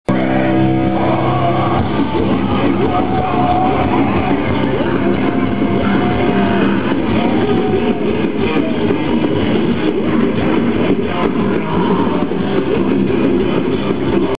Caliban Wall of Death - Summerbreeze'05
Wall of fucking Death bei Caliban auf dem Summerbreeze 05 in Abtsgmünd.